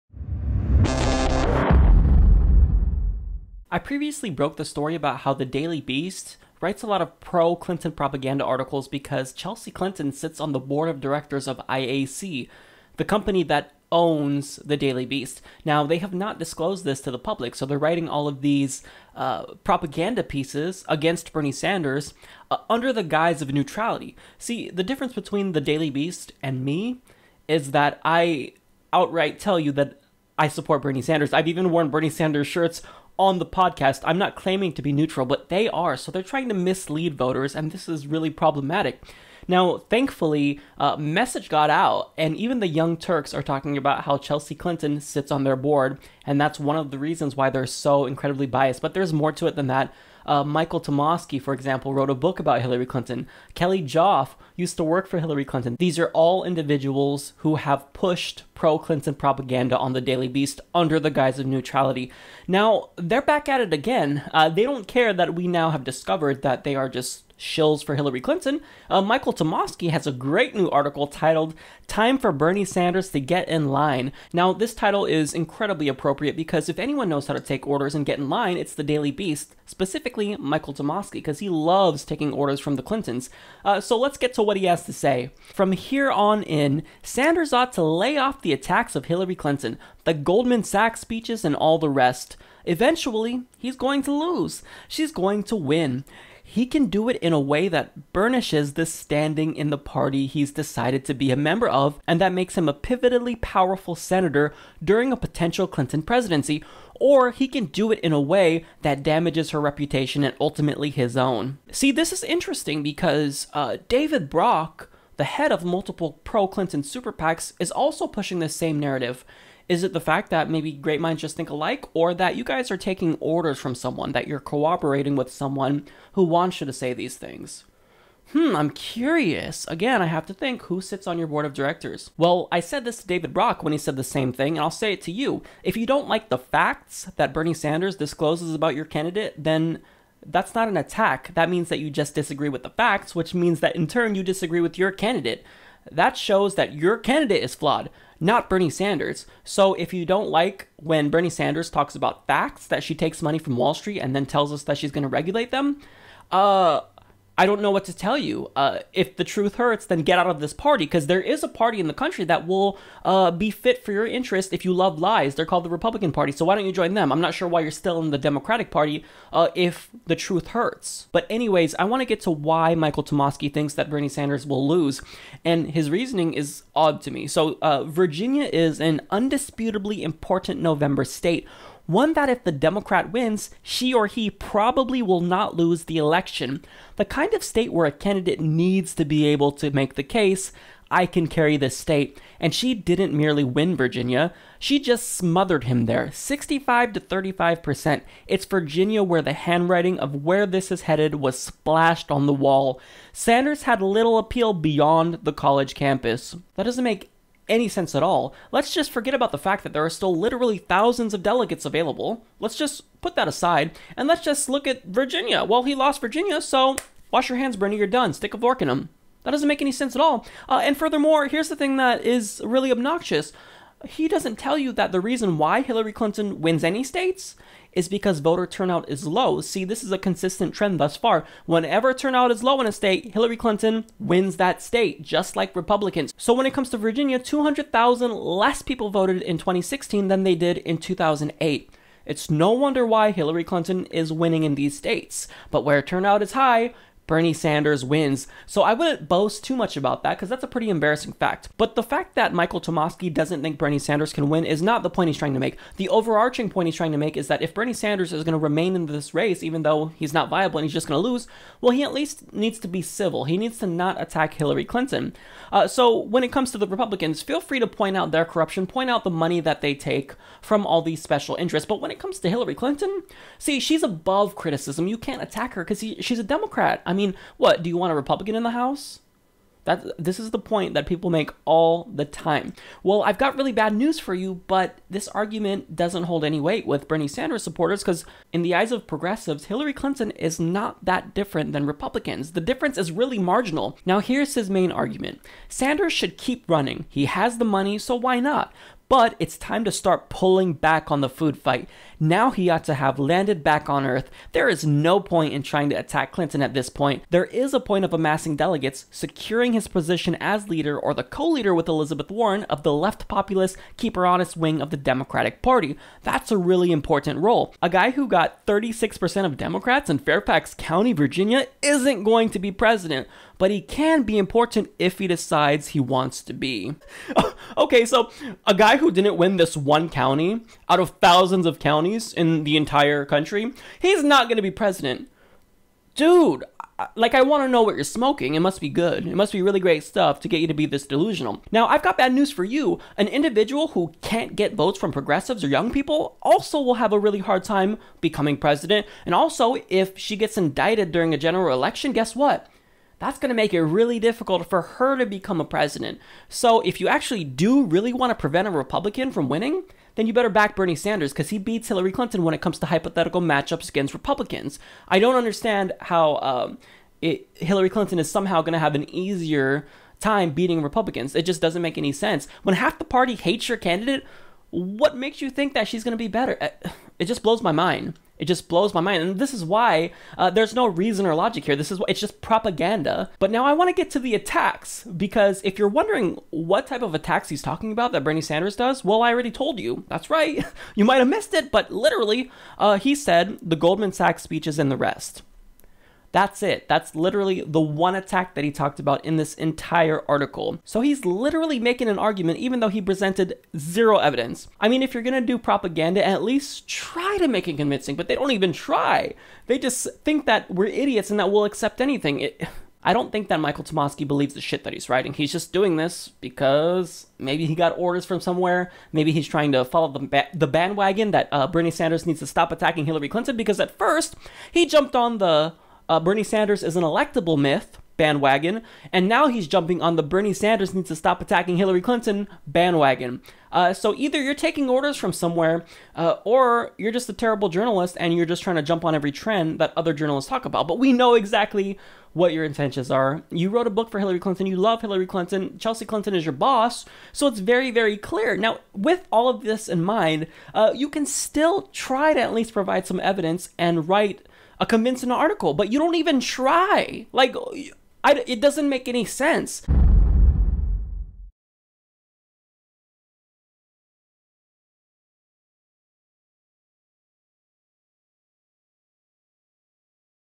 The Daily Beast's Propagandist Tirade Against Bernie Sanders Continues
The Daily Beast continued its tirade against Bernie Sanders with an all-new piece of propaganda. This time, they're pushing the "don't criticize" Hillary Clinton theme that Pro-Hillary Super PAC head David Brock is also pushing.